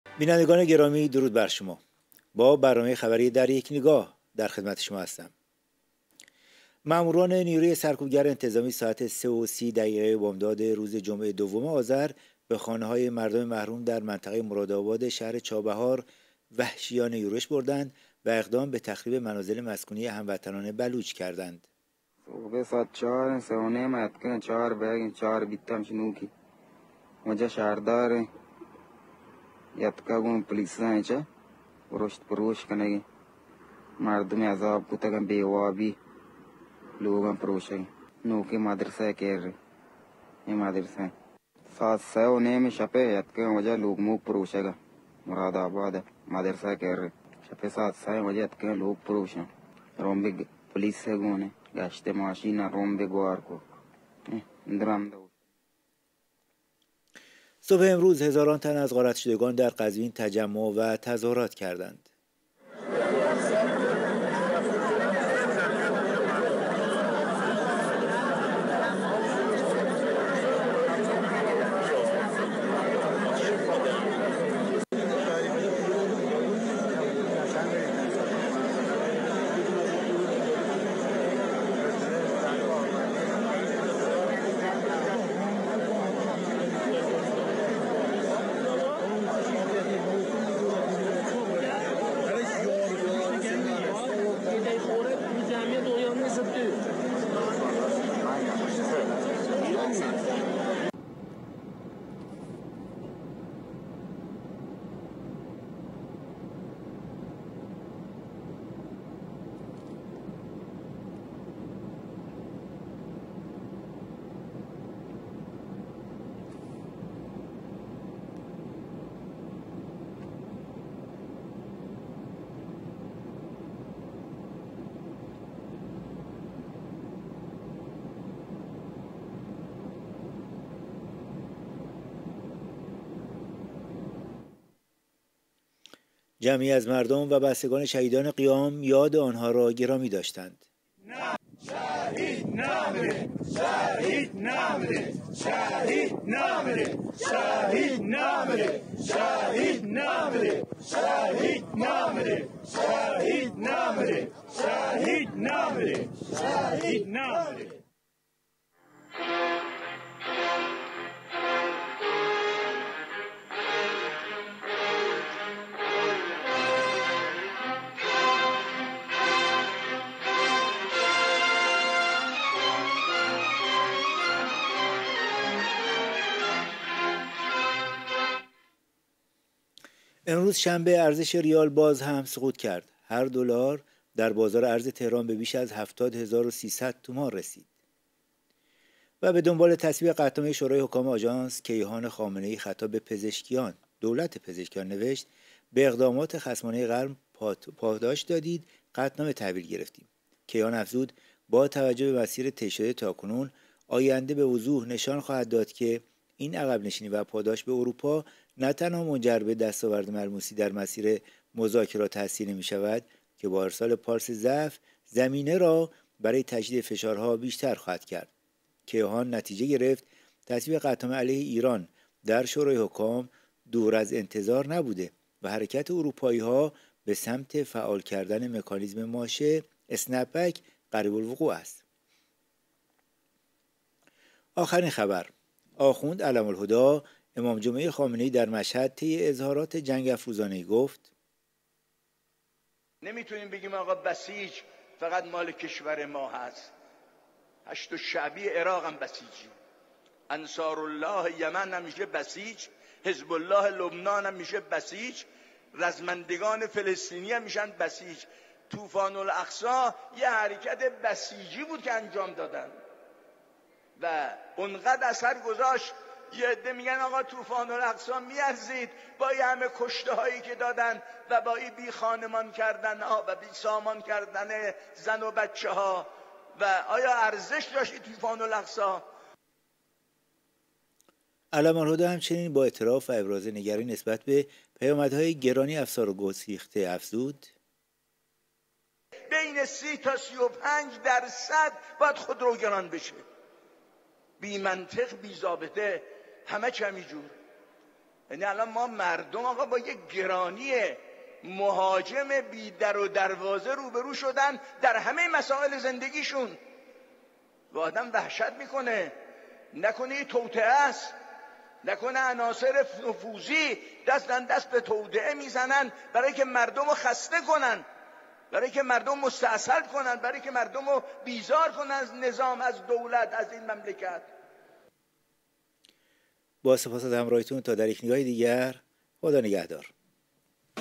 اخبار ایران و جهان در یک نگاه
تظاهرات مالباختگان قزوین علیه سیاستهای رژیم پلید آخوندی
مراسم بزرگداشت شهدای قبام آبان ۱۳۹۸
تخریب خانه های مردم محروم چابهار توسط مزدوران سرکوبگر آخوندی

سیمای آزادی ۳ آذر ۱۴۰۳